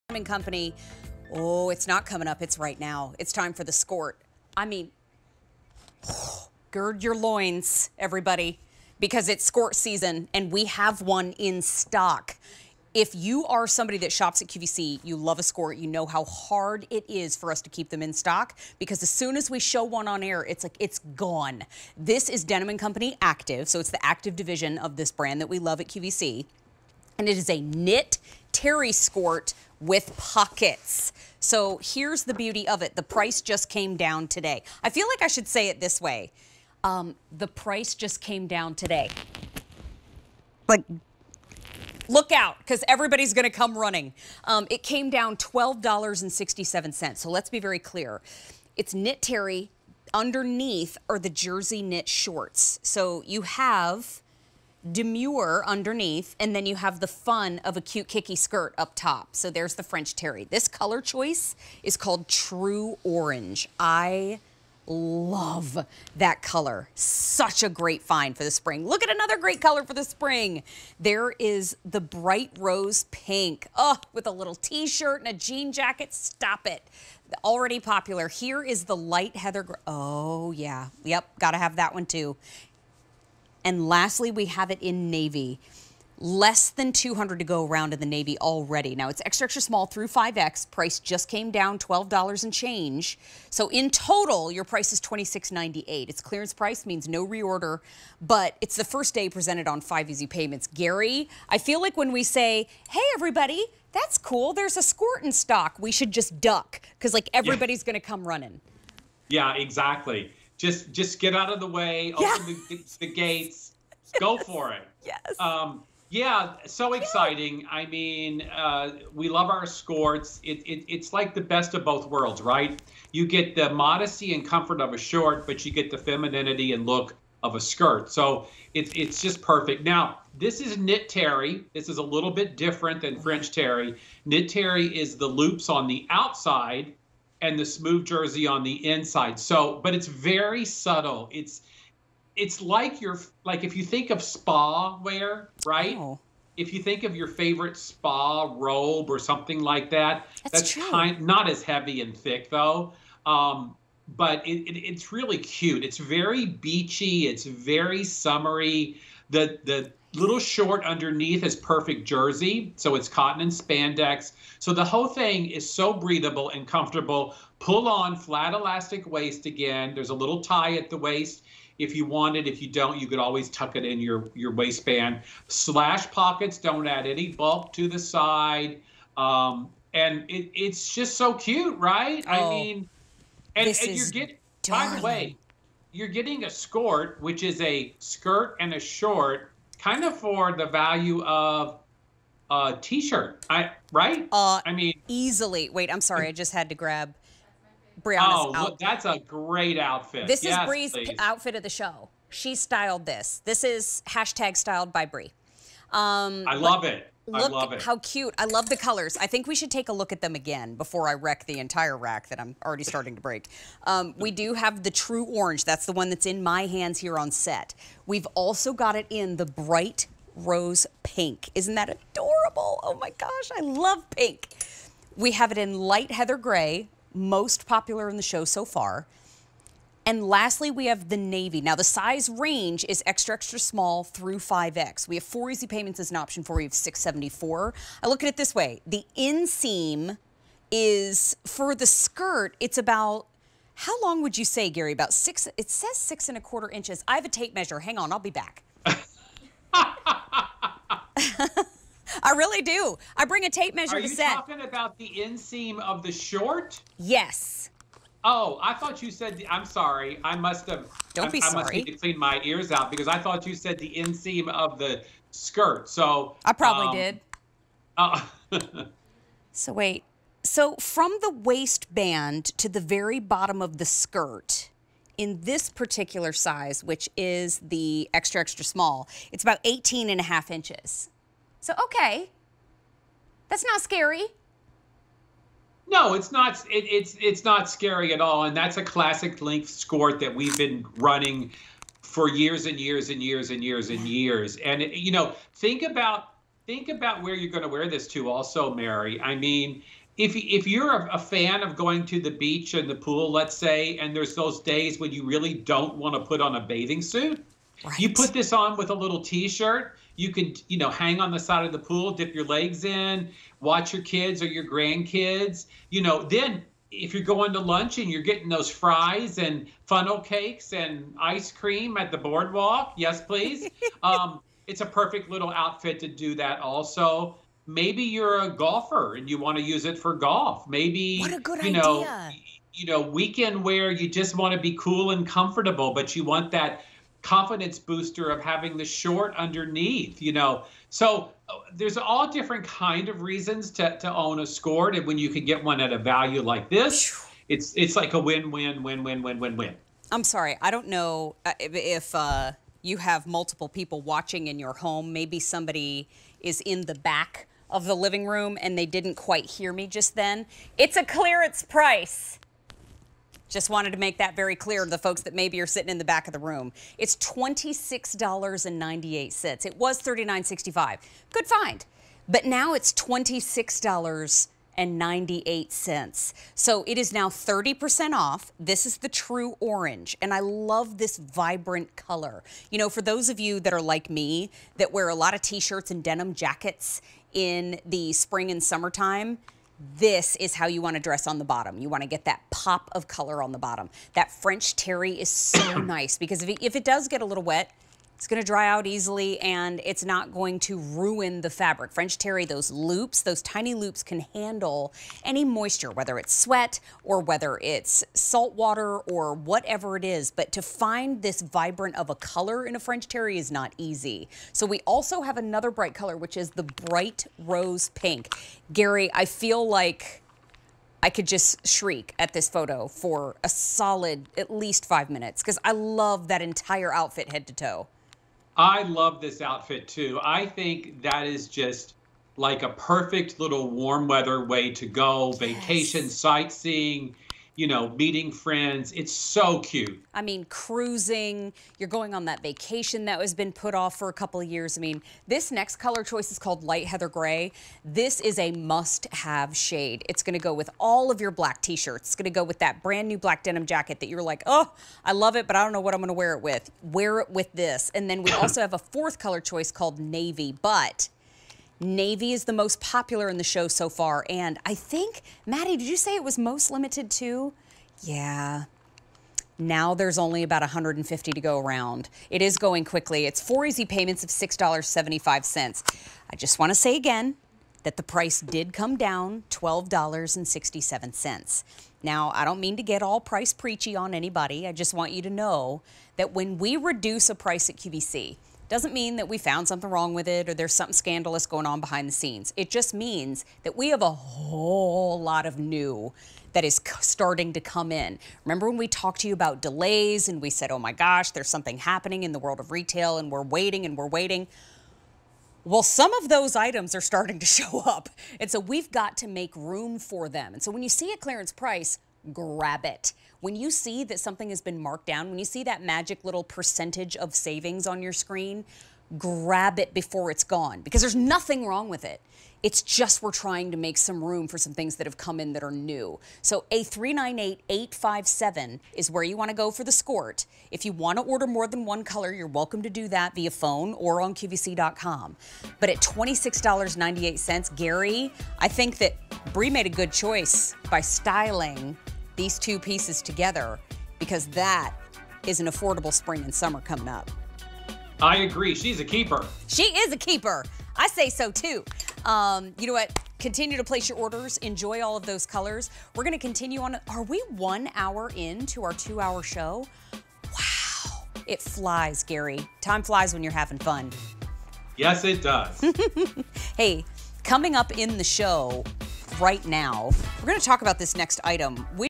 Denim & Co. Active Knit Terry Skort with Pockets on QVC
We are allll about that full coverage, and this skort (with pockets!) does it in a stylish knit terry skirt with soft jersey knit shorts. From Denim & Co.&reg; Fashions.